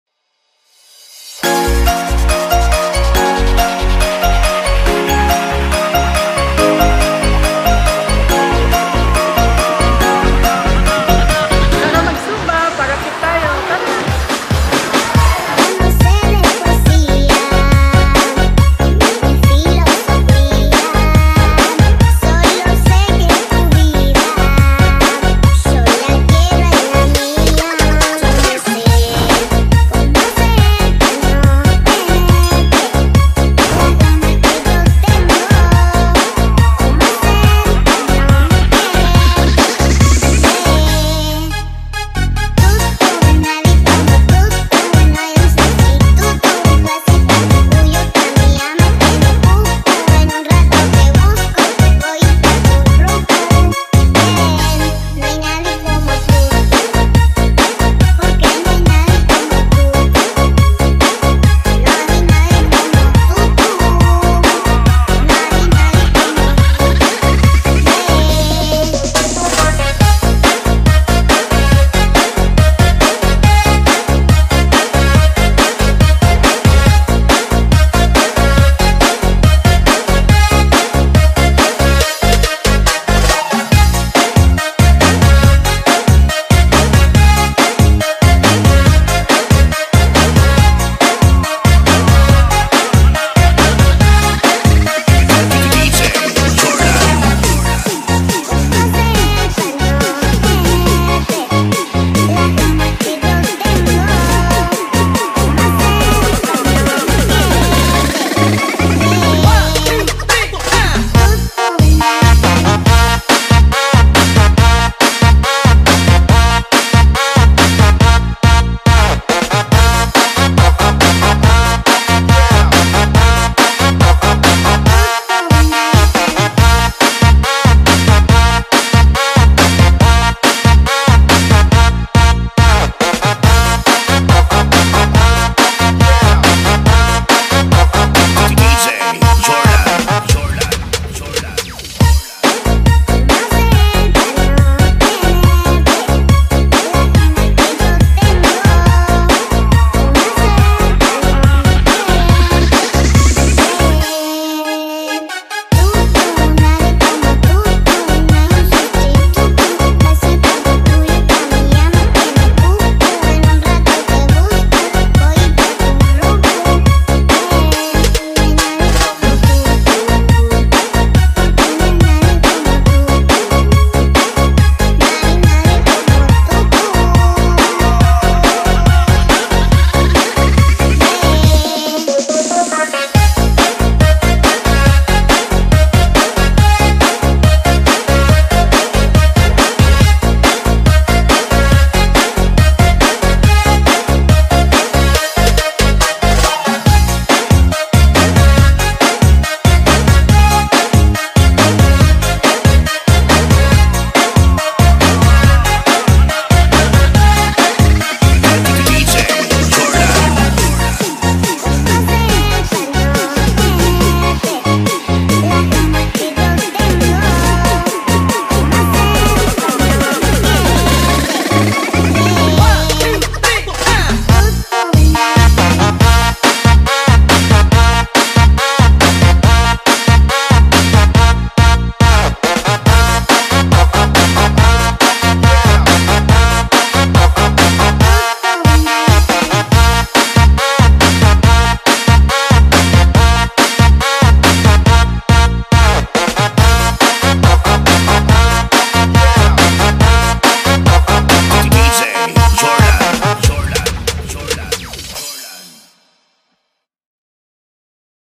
ZUMBAYORA: Para forda fit ang fersons ng Natividad
Kahit pagod ang lahat sa kanikanilang mga trabaho huwag pa din nating kalilimutan ang ating kalusugan. Halina't umindak at sumabay.